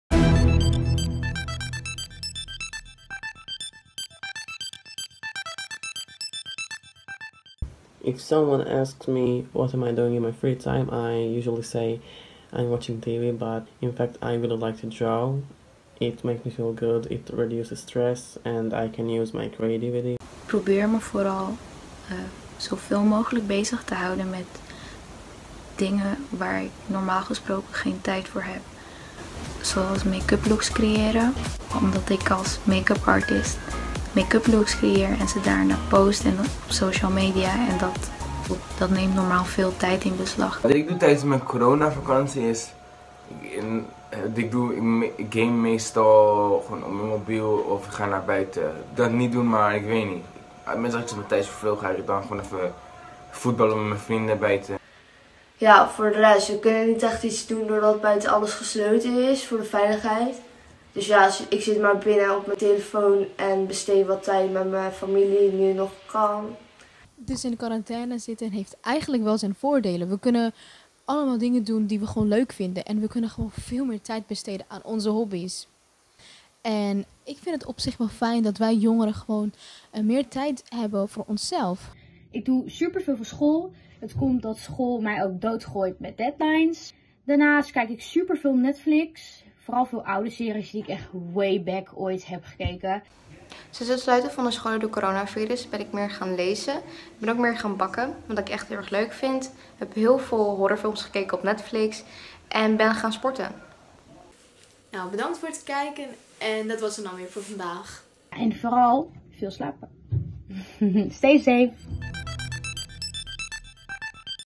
C6N reporter Mariah interviewt jongeren (ook in t buitenland) over Corona
C6N reporter Mariah interviewt jongeren (ook in t buitenland) met de vraag hoe zij hun Corona-tijd doorbrengen!